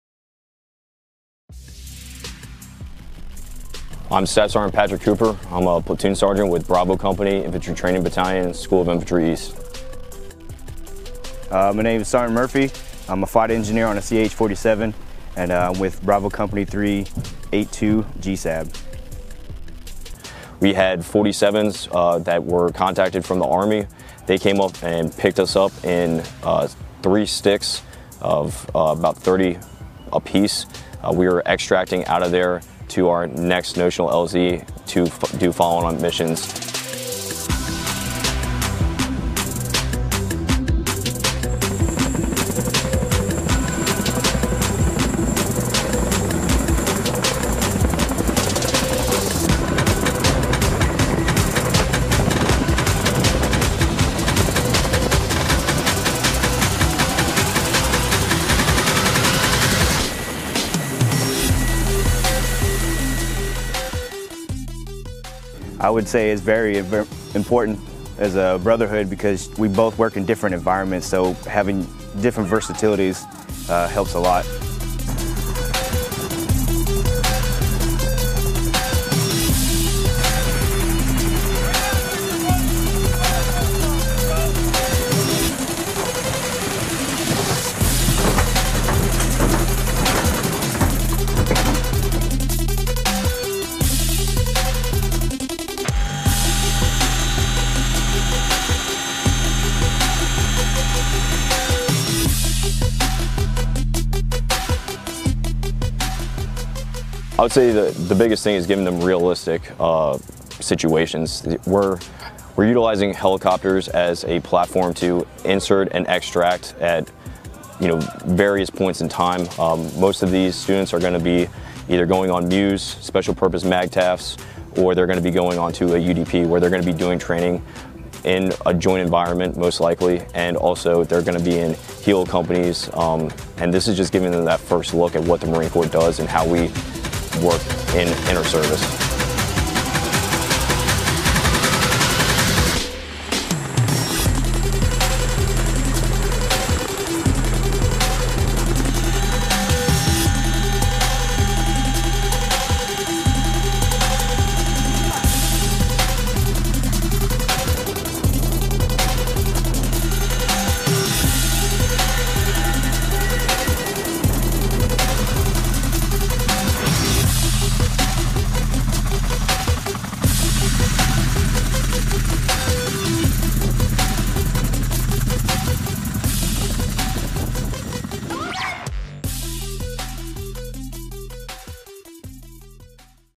Brothers in Arms
U.S. Marines assigned to Bravo Company, Infantry Training Battalion, School of Infantry-East, prepare to receive a class on a CH-47 Chinook helicopter during an aerial extraction/insertion exercise at Camp Lejeune, N.C., June 7, 2018. Entry-level Marines worked in concert with the U.S. Army's 82nd Airborne Division in order to introduce them to a wide-range of tactical advantages. (U.S. Marine Corps photo by Sgt. James R. Skelton)